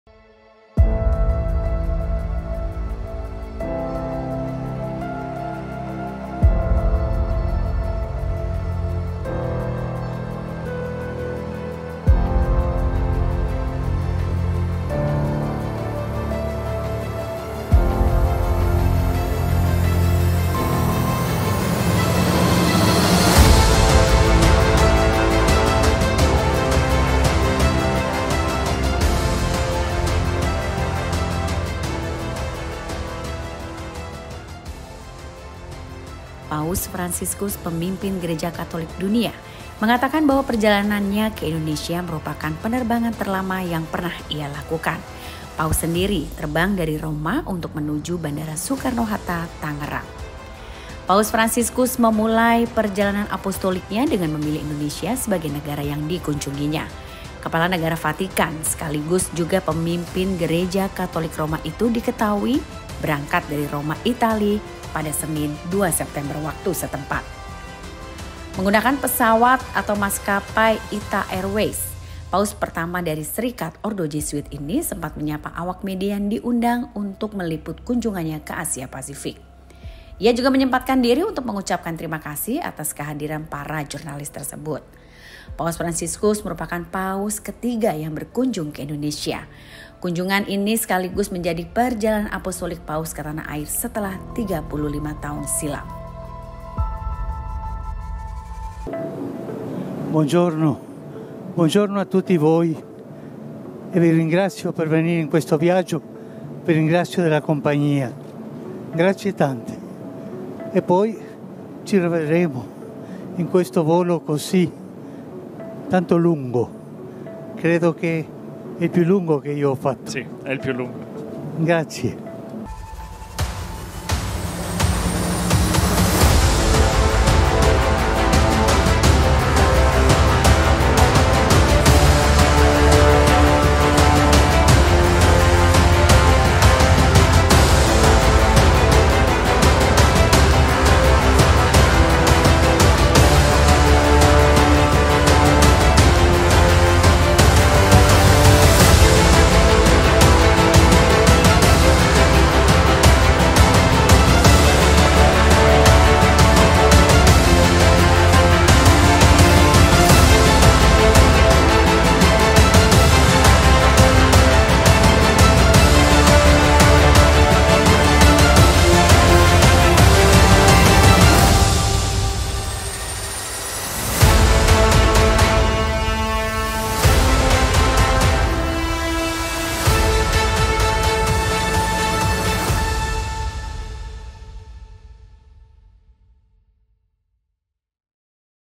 Cerita Paus Fransiskus: "Penerbangan Terlama yang Pernah Saya Lakukan"
WARTAKOTALIVE.COM, Jakarta – Paus Fransiskus Pemimpin Gereja Katolik Dunia, mengatakan bahwa perjalanan ke Indonesia merupakan penerbangan terlama yang pernah ia lakukan.
Paus sendiri terbang dari Roma untuk menuju Bandara Soekarno-Hatta, Tangerang.
Paus Fransiskus memulai perjalanan apostoliknya dengan memilih Indonesia sebagai negara yang dikunjunginya.

Video Editor: Luttex